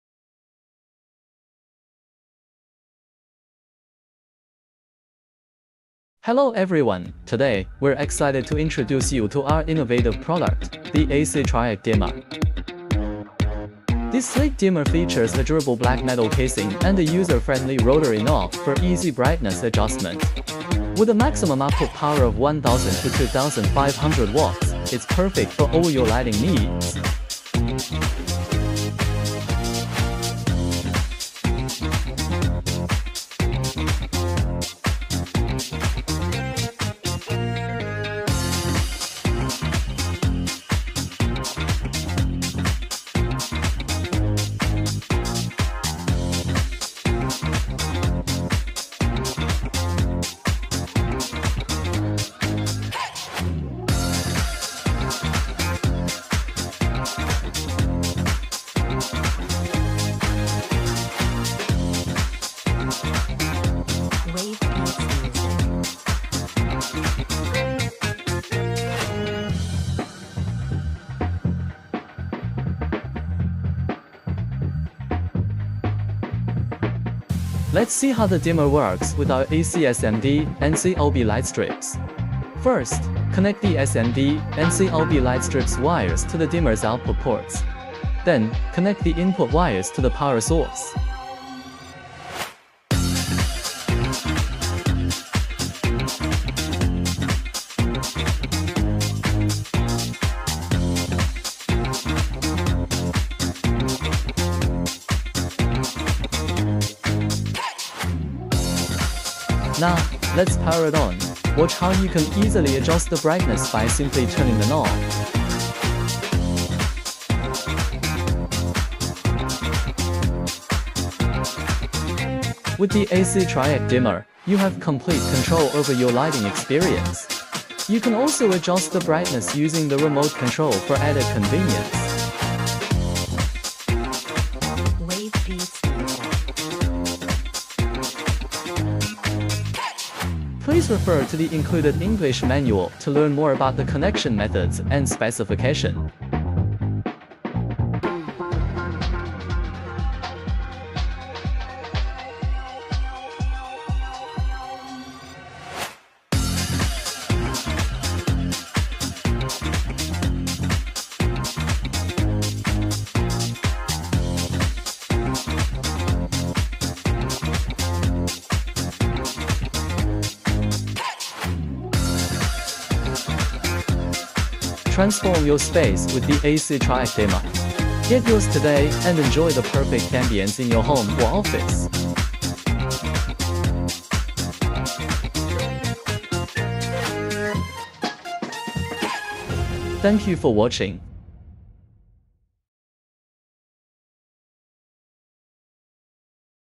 How to Adjust AC LED Strip Brightness with a Triac LED Dimmer | Step-by-Step Guide
In today's video, we will explore how to adjust the brightness of AC LED strips using a triac LED dimmer.

We will be using a high-quality dimmer with a black iron housing, featuring a convenient rotary adjustment knob for easy operation.

This dimmer supports a maximum output power of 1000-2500 watts and comes with an English instruction manual for understanding connection methods and specifications.

We will demonstrate the connection process using two types of AC LED strips: SMD and COB LED strips.

Watch as we connect the SMD strip to the dimmer output, attach the input wires, and power it on.

You’ll see how the brightness can be easily adjusted—both manually with the knob and remotely via the included remote control.

We will also perform a voltage test with a voltage tester to show that the input voltage is 220V, while the output voltage varies according to your dimming preferences.

Join us to learn how to effectively use a triac LED dimmer for your lighting projects and make your environment more versatile with adjustable brightness!

Any more information about this strip light, pls check this